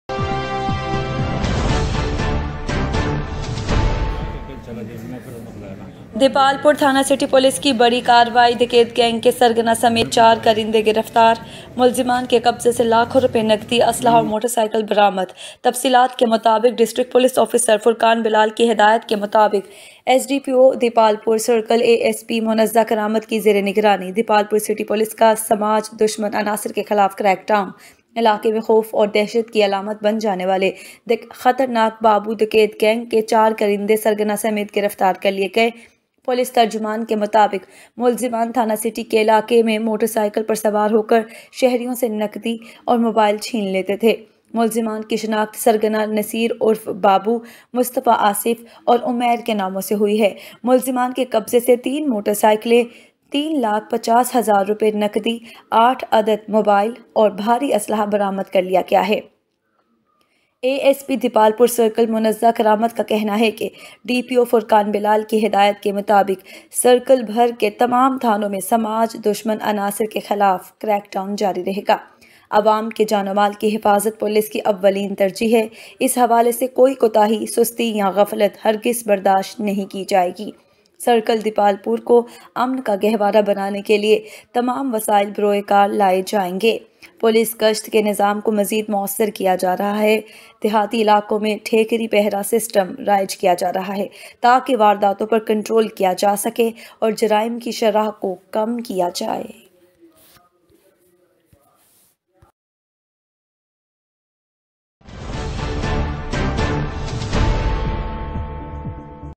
دیپالپور : تھانہ سٹی پولیس کی بڑی کاروائی ڈکیت گینگ کے سرغنہ سمیت چار کارندے گرفتار
دیپالپور ،،، تھانہ سٹی پولیس کی بڑی کاروائی ڈکیت گینگ کے سرغنہ سمیت چار کارندے گرفتار،ملزمان کے قبضہ سے لاکھوں روپے نقدی اسلحہ اور موٹرسائکلیں برآمد تفصیلات کے مطابق ڈسٹرکٹ پولیس آفیسر فرقان بلال کی ہدائیت کے مطاق ایس ڈی پی او دیپالپور سرکل اے ایس پی منزہ کرامت کی زیرنگرانی دیپالپور سٹی پولیس کا سماج دشمن عناصر کے خلاف کریک ڈاون علاقہ میں خوف اور دہشت کی علامت بن۔جانے والے خطرناک بابو ڈکیت گینگ کے چار کارندے سرغنہ سمیت گرفتار کر لئے گئے پولیس ترجمان کے مطابق ملزمان تھانہ سٹی کے علاقہ میں موٹر سائیکل پر سوار ہو کر شہریوں سے نقدی اور موبائل چھین لیتے تھے ملزمان کی شناخت سرغنہ نصیر عرف بابو ،مصطفی، آصف اور عمیر کے ناموں سے ہوئی ہے ملزمان کے قبضہ سے تین موٹر سائکلیں 350000روپے نقدی 8عدد موبائل اور بھاری اسلحہ برآمد کر لیا گیا ہے اے ایس پی دیپالپور سرکل منزہ کرامت کا کہنا ہے کہ ڈی پی او فرقان بلال کی ہدائیت کے مطابق سرکل بھر کے تمام تھانوں میں سماج دشمن عناصر کے خلاف کریں ڈاون جاری رہے گا عوام کے جان ومال کی حفاظت پولیس کی اولین ترجیح ہے اس حوالے سے کوئی کوتاہی سستی یا غفلت ہرگز برداشت نہیں کی۔جائے گی سرکل دیپالپور کو امن کا گہوارہ بنانے کے لئے تمام وسائل بروئے کار لائے جائیں گے پولیس گشت کے نظام کو مزید موثر کیا جا رہا ہے دیہاتی علاقوں میں ٹھیکری پہرا سسٹم رائج کیا جا رہا ہے تاکہ وارداتوں پر کنٹرول کیا جا سکے اور جرائم کی شرح کو کم کیا جائے۔۔

Pakistan's First Mega News 24 (4k) Ultra HD
Getting All News From Allover The World
Breaking news,District News,Local News,Live News Ultra HD